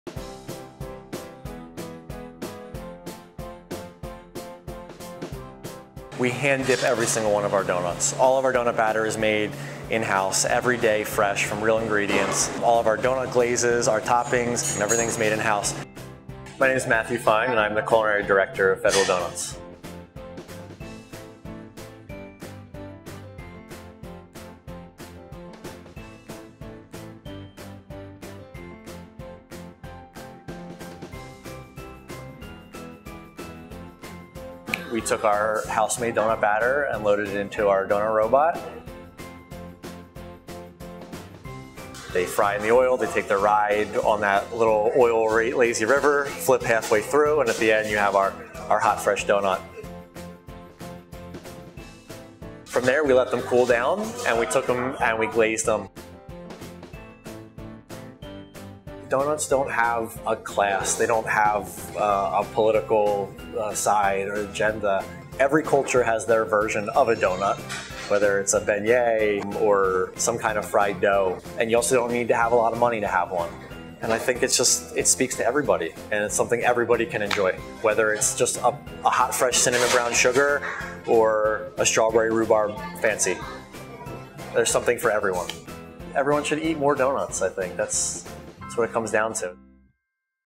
Hot off the Donut Robot: Inside Federal Donuts
For National Doughnut Day, Federal Donuts' Matthew Fein took us behind the scenes and into the kitchen of this Philly favorite.

KRISTEN BALDERAS/STAFF